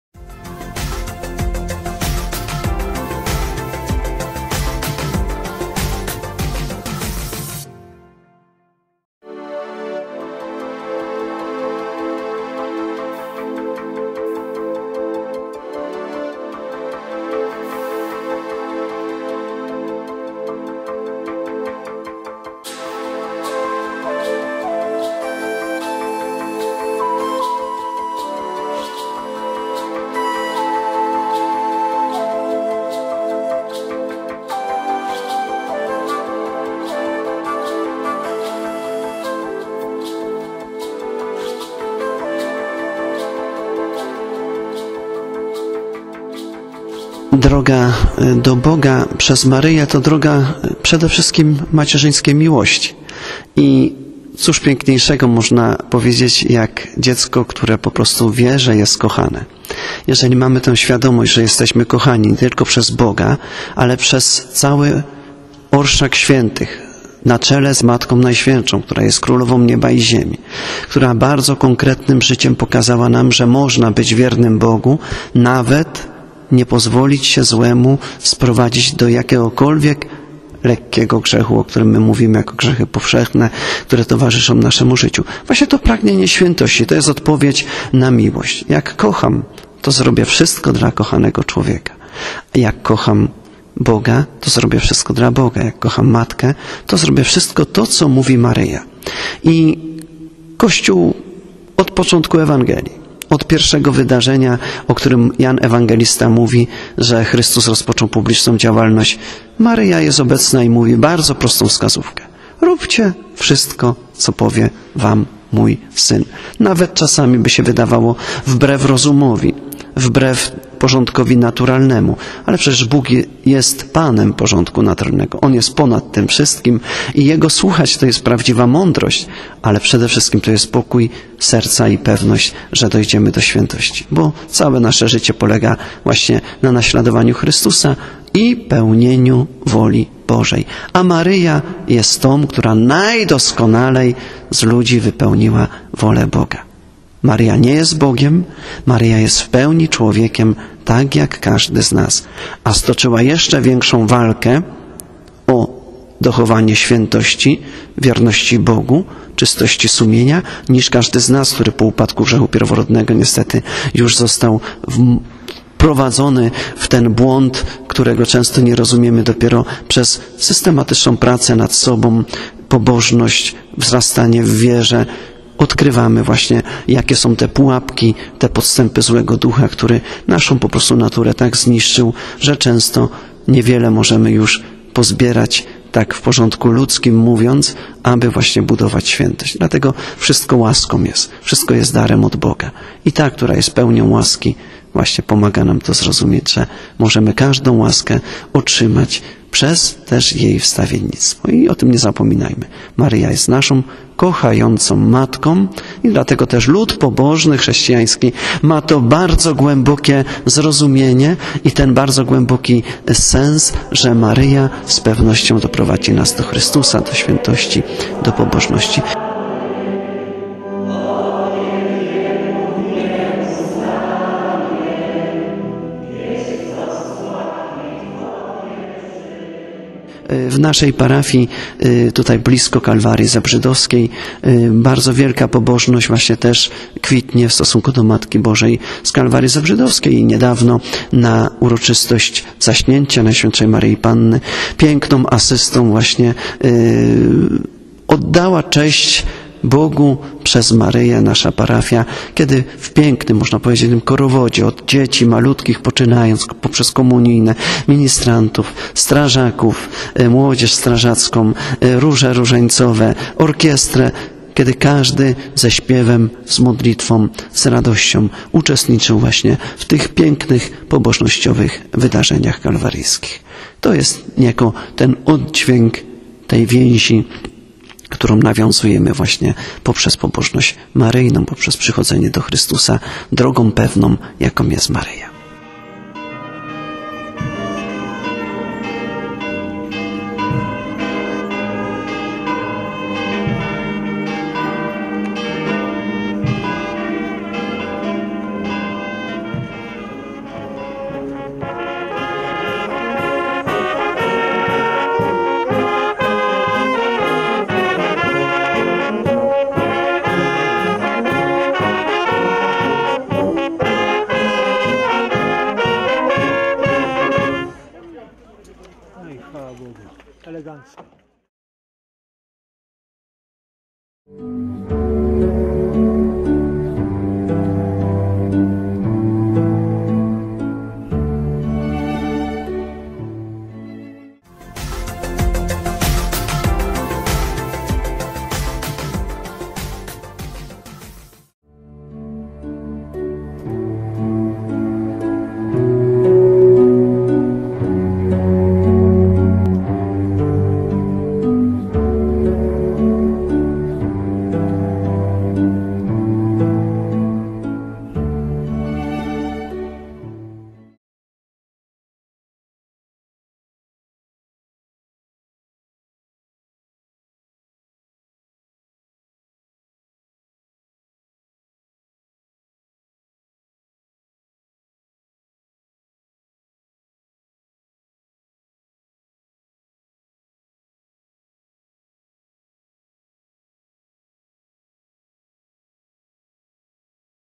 Koronacja Obrazu MB Pocieszenia w Budzowie; TvM
TELEWIZJA MAKSYMILIAN: Macierzyńska Droga Miłości, W przeddzień Koronacji Obrazu Matki Bożej Pocieszenia w Budzowie/Archidiecezja Krakowska/, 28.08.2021 r.